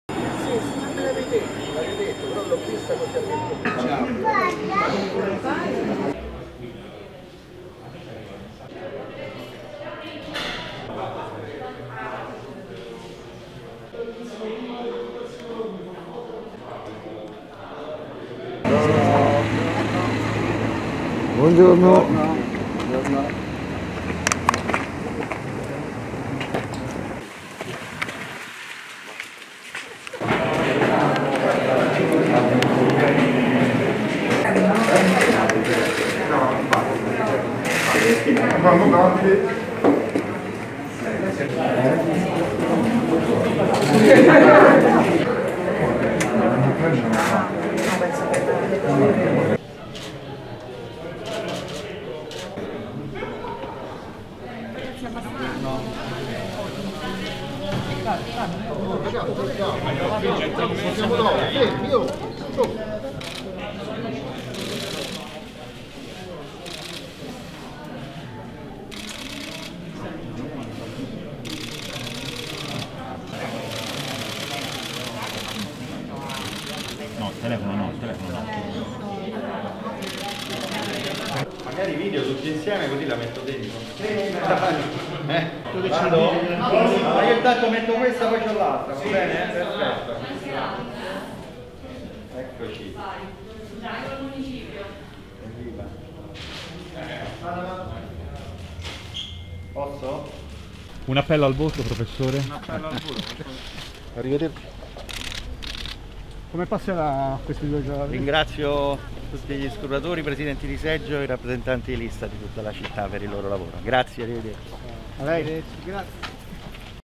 Roma, Gualtieri in fila al seggio: "Grazie a presidenti, scrutatori e rappresentanti di lista"
Il candidato sindaco di Roma del centrosinistra Roberto Gualtieri ha votato all'I.t.c. Federico Caffè intorno alle ore 11, nel quartiere di Monteverde. Accompagnato dalla scorta e da alcuni collaboratori, l'ex ministro dell'Economia ha atteso il suo turno in fila prima di entrare nella sezione 1427 per esprimere il proprio voto. "Ringrazio tutti gli scrutatori, i presidenti di seggio e i rappresentanti di lista di tutta la città per il loro lavoro", si è limitato a dire il candidato ai cronisti presenti dopo aver votato .  .di Francesco Giovannetti